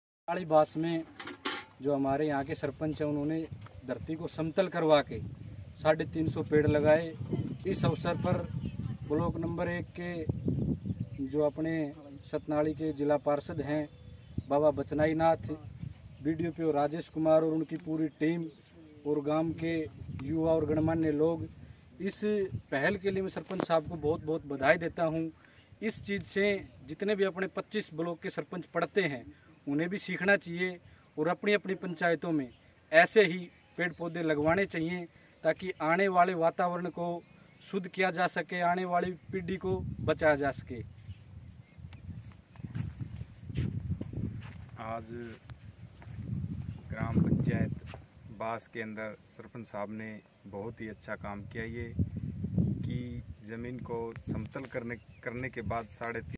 सतनाली बास में आज 351पेड़ लगाए गए फल दार और छाया दार और 3 त्रिवेणी लगाई है सरपंच कर्णपाल जिला पार्षद
सरपंच वीरपाल, जिला पार्षद बचराई नाथ, सतनाली ब्लॉक का पूरा स्टॉफ के साथ पेड़ रोपण किया है